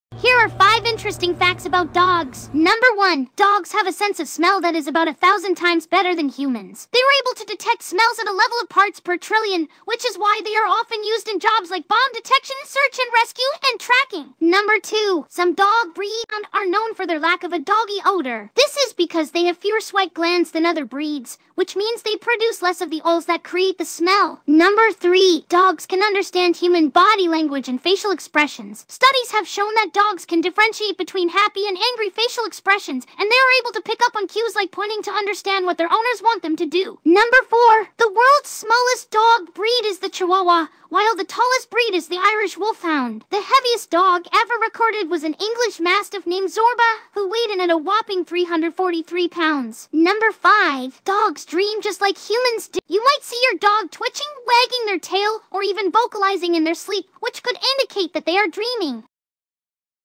Do you love dogs? If you do, here are 5 interesting facts about dogs.
Do you love dogs? If you do, here are 5 interesting facts about dogs. You can join the conversation by liking and commenting on this video.

Transcript:
1. Dogs have a sense of smell that is about 1,000 times better than humans. They are able to detect smells at a level of parts per trillion, which is why they are often used in jobs like bomb detection, search and rescue, and tracking.

2. Some dog breeds, such as the Basenji and the Afghan Hound, are known for their lack of a "doggy" odor. This is because they have fewer sweat glands than other breeds, which means they produce less of the oils that create the smell.

3. Dogs can understand human body language and facial expressions. Studies have shown that dogs can differentiate between happy and angry facial expressions, and they are able to pick up on cues like pointing to understand what their owners want them to do.

4. The world's smallest dog breed is the Chihuahua, while the tallest breed is the Irish Wolfhound. The heaviest dog ever recorded was an English Mastiff named Zorba, who weighed in at a whopping 343 pounds.

5. Dogs dream just like humans do. When dogs are in a deep sleep, they enter into the REM (rapid eye movement) phase of sleep, which is when dreaming occurs. You might see your dog twitching, wagging their tail, or even vocalizing in their sleep, which could indicate that they are dreaming.